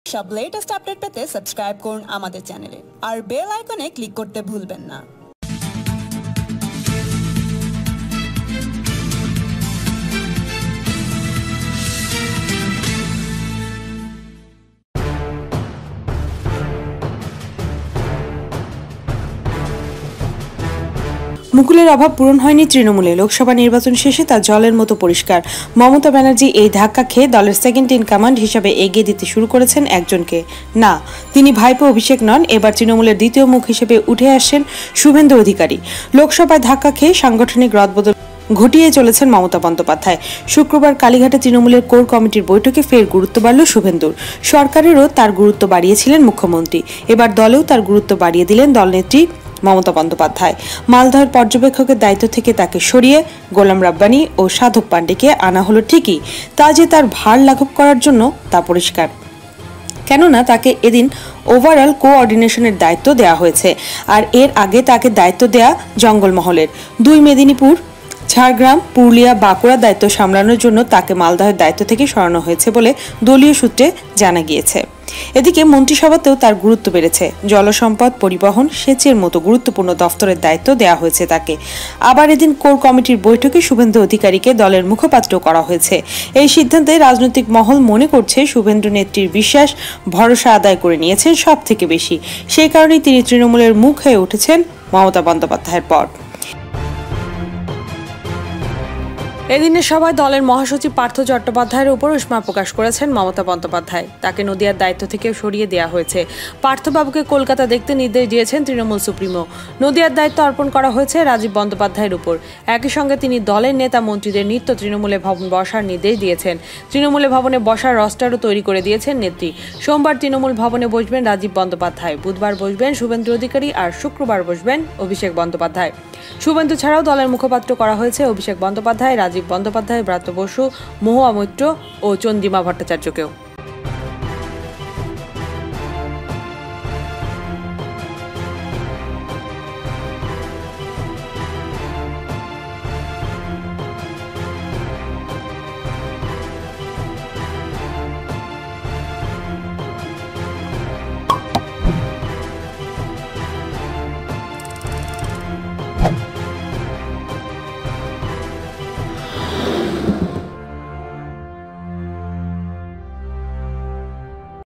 মমতার বড় সিদ্ধান্ত ! এবার তৃণমূলের দ্বিতীয় মুখ হিসেবে উঠে আসছেন শুভেন্দু অধিকারী!
মুকুলের অভাব পূরণ হয়নি তৃণমূলে। লোকসভা নির্বাচন শেষে তা জলের মতো পরিষ্কার। মমতা ব্যানার্জী এই  ধাক্কা খেয়ে দলে সেকেন্ড ইন কম্যান্ড হিসেবে এগিয়ে দিতে শুরু করেছেন একজনকে। না, তিনি ভাইপো অভিষেক নন, এবার তৃণমূলের দ্বিতীয় মুখ হিসেবে উঠে আসছেন শুভেন্দু অধিকারী।

লোকসভায় ধাক্কা খেয়ে সাংগঠনিক রদবদল ঘটিয়েই চলেছেন মমতা বন্দ্যোপাধ্যায়। শুক্রবার কালীঘাটে তৃণমূলের কোর কমিটির বৈঠকে ফের গুরুত্ব বাড়ল শুভেন্দুর। সরকারেও তাঁর গুরুত্ব বাড়িয়েছিলেন মুখ্যমন্ত্রী। এবার দলেও তাঁর গুরুত্ব বাড়িয়ে দিলেন দলনেত্রী মমতা বন্দ্যোপাধ্যায়। মালদহের পর্যবেক্ষকের দায়িত্ব থেকে তাঁকে সরিয়ে গোলাম রব্বানি ও সাধন পান্ডেকে আনা হল ঠিকই, তা যে তাঁর ভার লাঘব করার জন্য, তা পরিষ্কার। কেননা তাঁকে এদিন ওভার-অল কো-অর্ডিনেশনের দায়িত্ব দেওয়া হয়েছে। আর এর আগে তাঁকে দায়িত্ব দেওয়া জঙ্গলমহলের। দুই মেদিনীপুর, ঝাড়গ্রাম, পুরুলিয়া, বাঁকুড়া দায়িত্ব সামলানোর জন্যই তাঁকে মালদহের দায়িত্ব থেকে সরানো হয়েছে বলে দলীয় সূত্রে জানা গিয়েছে।

এদিকে মন্ত্রিসভাতেও তাঁর গুরুত্ব বেড়েছে। জলসম্পদ, পরিবহন, সেচের মতো গুরুত্বপূর্ণ দফতরের দায়িত্ব দেওয়া হয়েছে তাঁকে। আবার এদিন কোর কমিটির বৈঠকে শুভেন্দু অধিকারীকে দলের মুখপাত্রও করা হয়েছে। এই সিদ্ধান্তেই রাজনৈতিক মহল মনে করছে শুভেন্দু নেত্রীর বিশ্বাস, ভরসা আদায় করে নিয়েছেন সবথেকে বেশি। সেই কারণেই তিনি তৃণমূলের মুখ হয়ে উঠছেন মমতা বন্দ্যোপাধ্যায়ের পর।